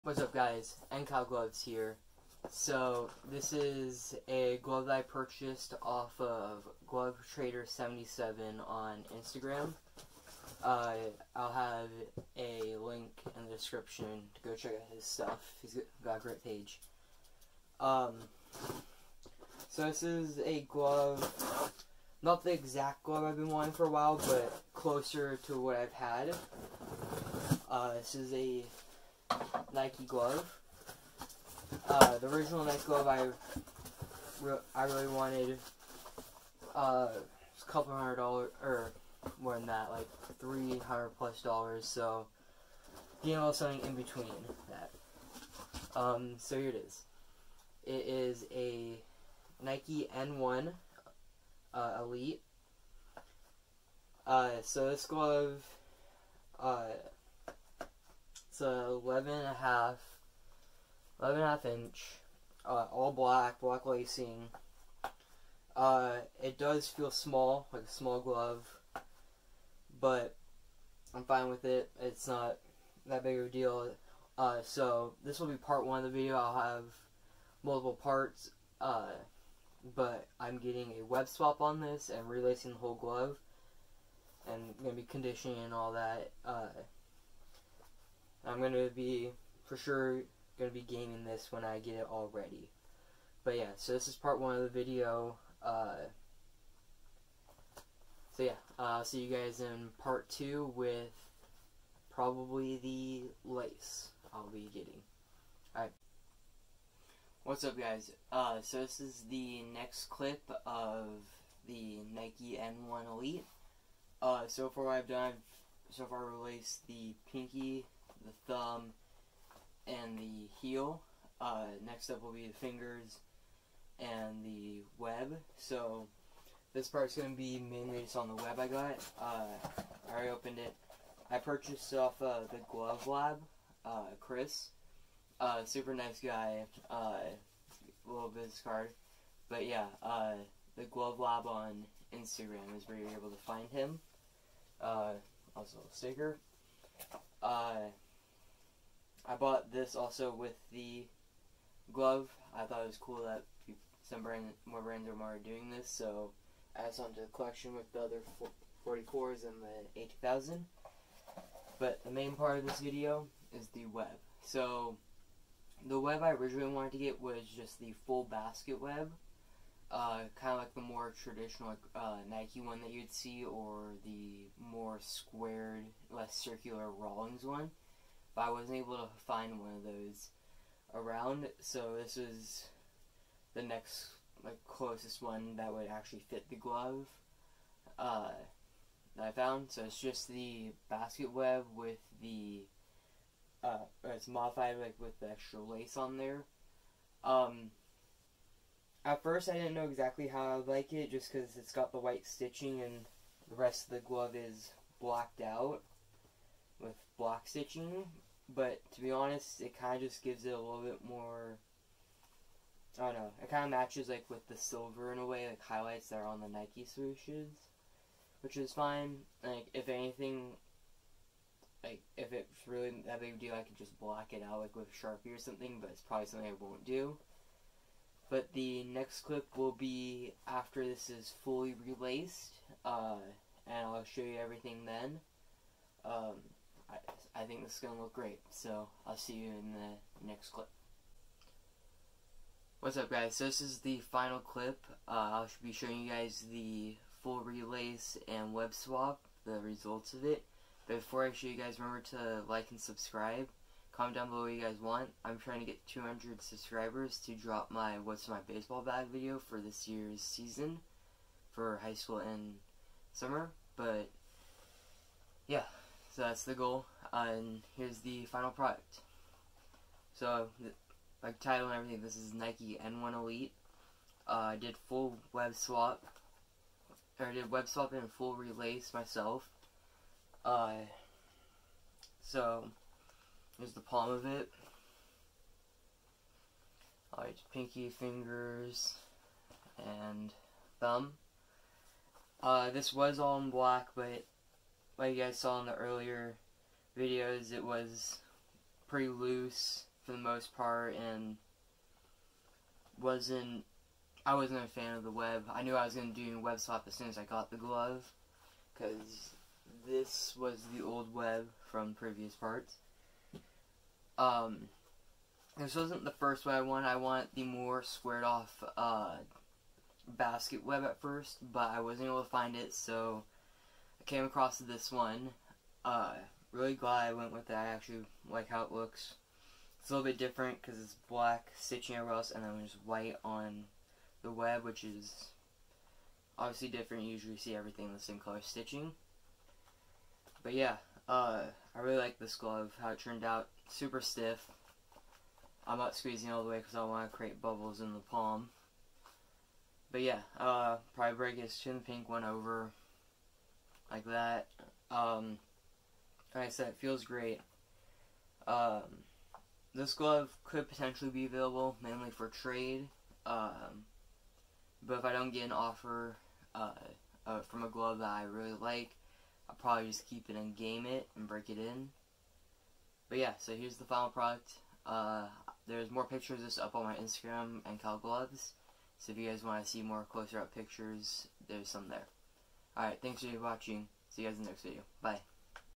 Nike N1 Elite Glove Mail Day + Full Glove Relace and Web Swap!
Also comment any questions or video you want to see!